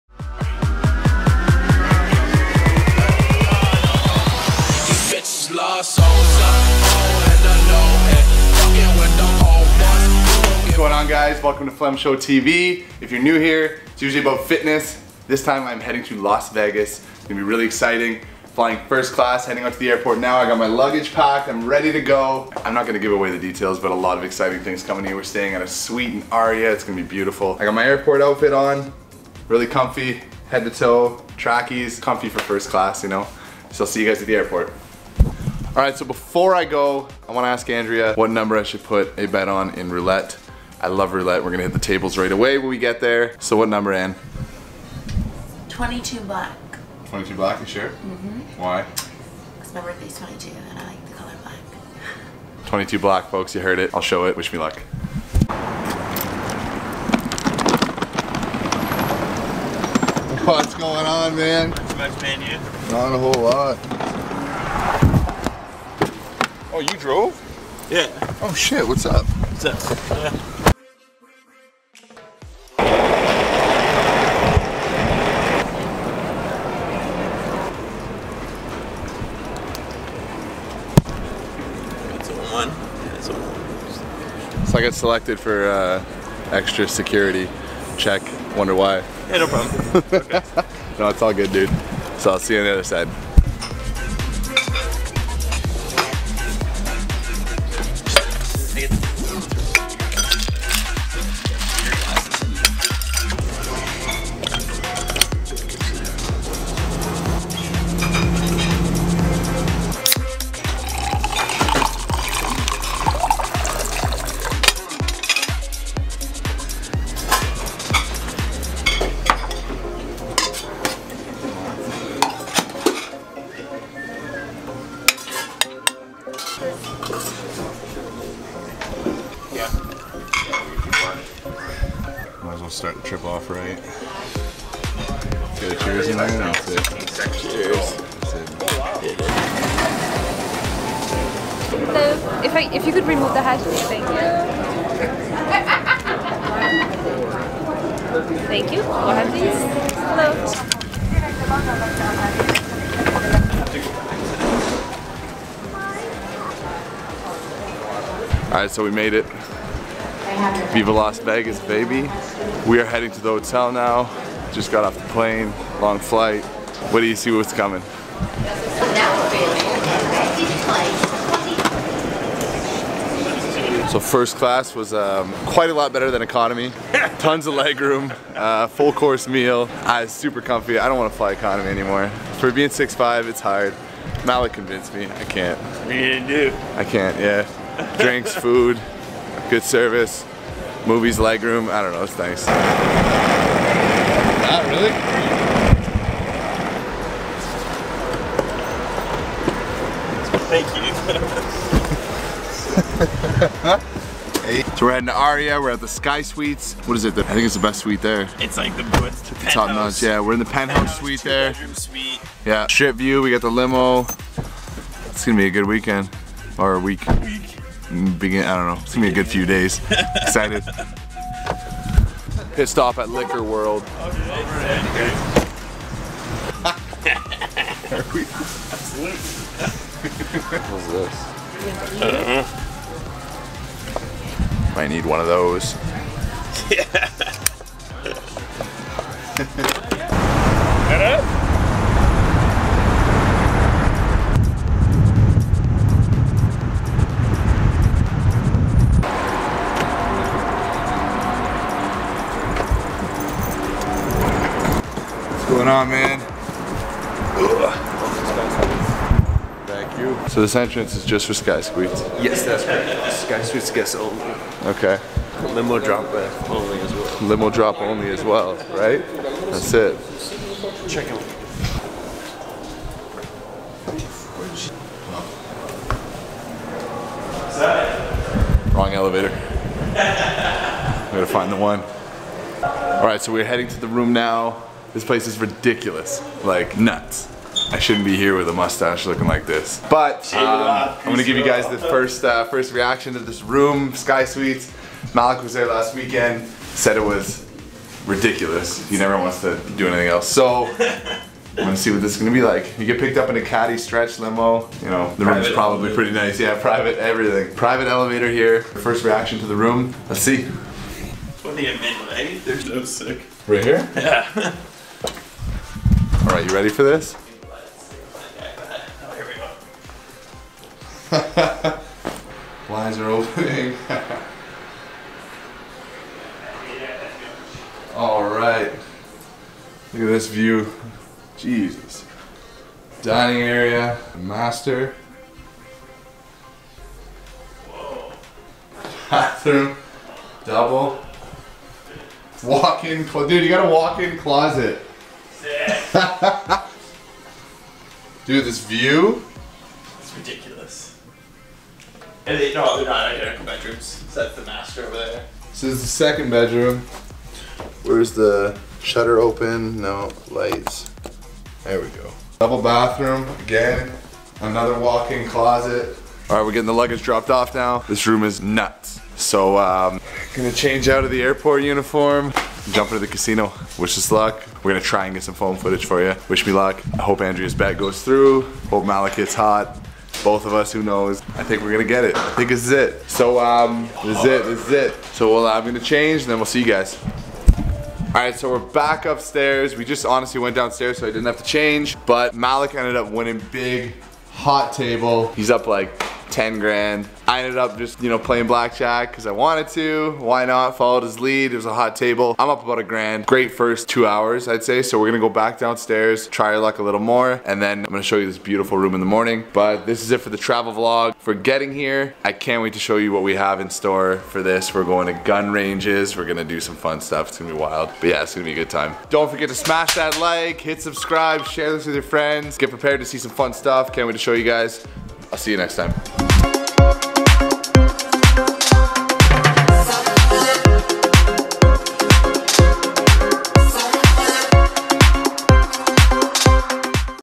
VIVA LAS VEGAS VLOGS | EP. 1 | $15,000 BLACKJACK WIN
FAM! Episode 1 from my CRAZY trip to Las Vegas 😜 wait until you see what I have in store for you on the NEXT one...🤩